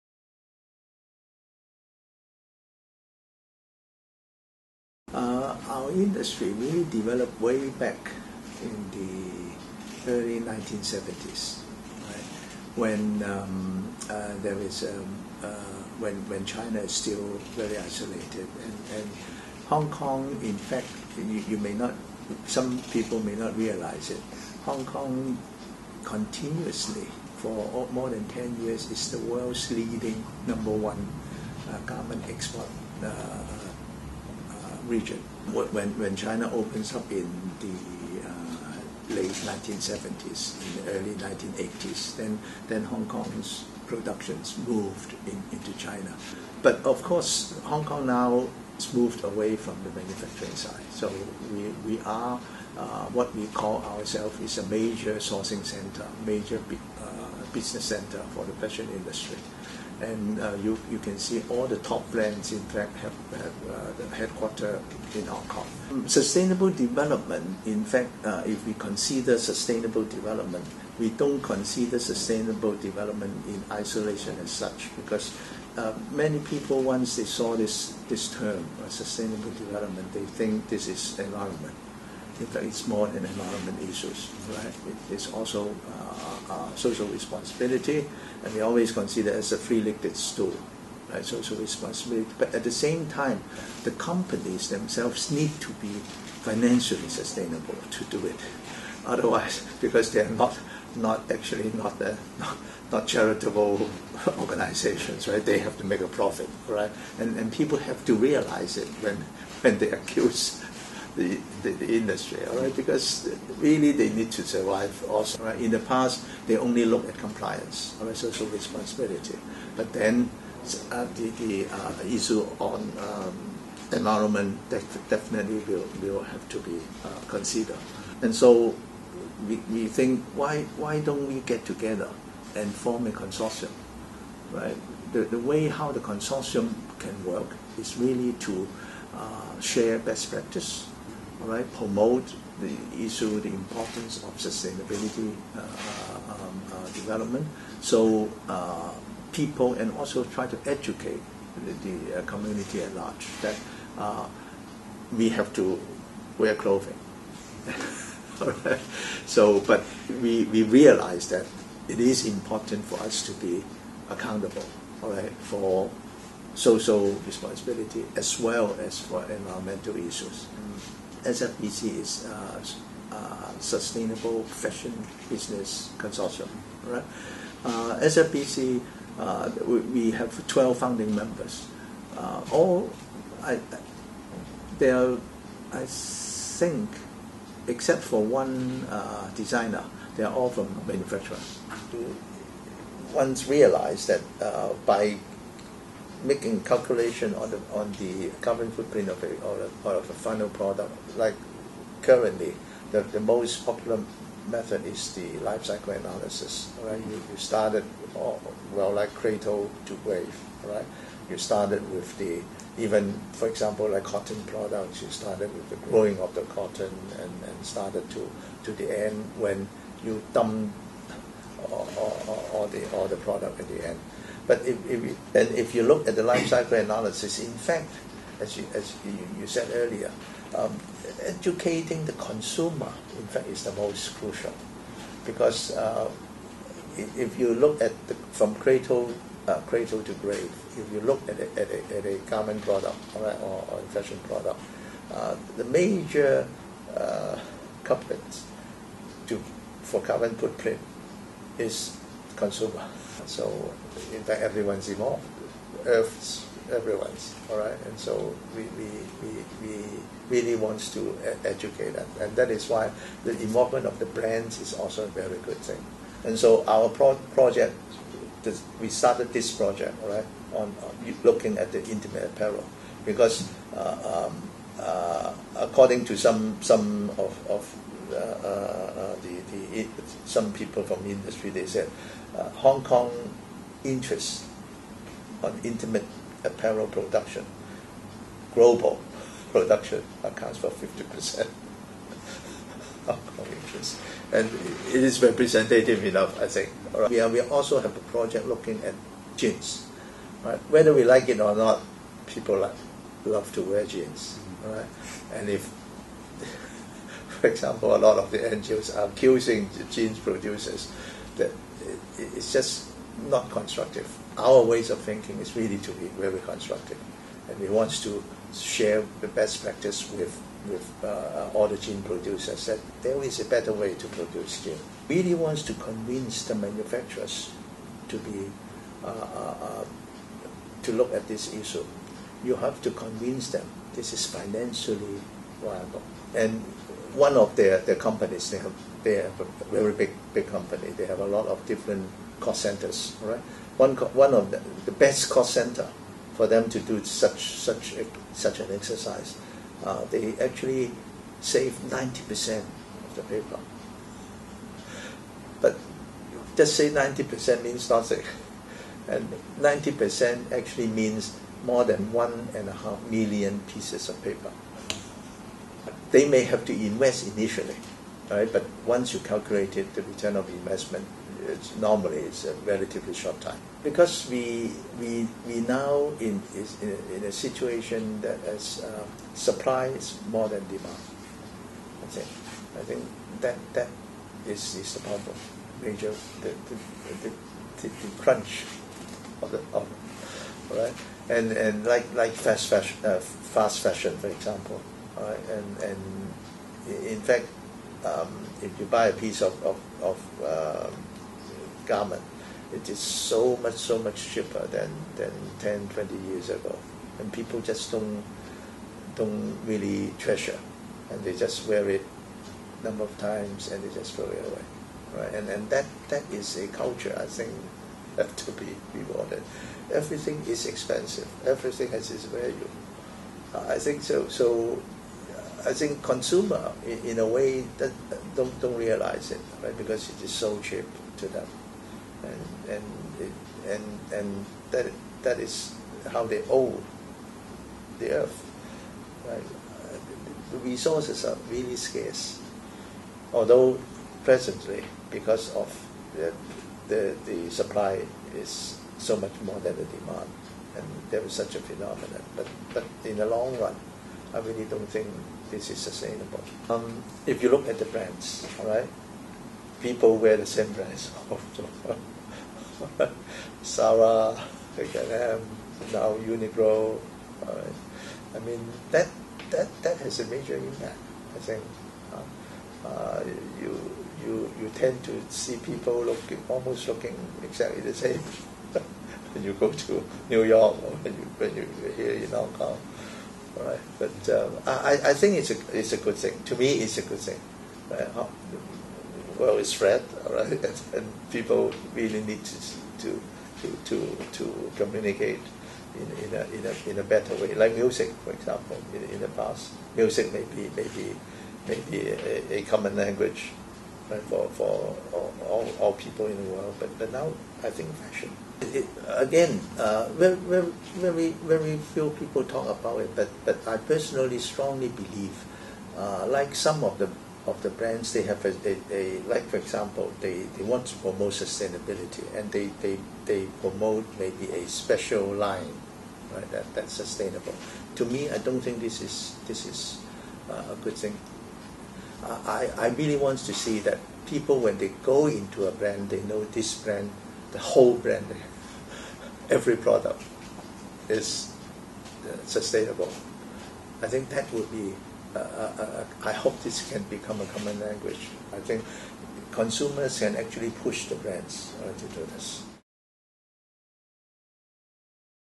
Philip Yeung - What Do I Owe You?
Professor Philp Yeung is the Executive Director of the Clothing Industry Training Authority in Hong Kong. In 2008, he initiated the Sustainable Fashion Business Consortium with a dozen major fashion companies based in Hong Kong. He briefly outlines the development of the fashion industry in the former British exclave and greater China, and explains the challenges of sustainability, advocacy, and consumer awareness within the realm of fashion. Professor Yeung stresses the consumers' importance and power and the necessity to change their consumption behavior. His hope is that fashion will have an impact similar to music, as a uniting medium across borders.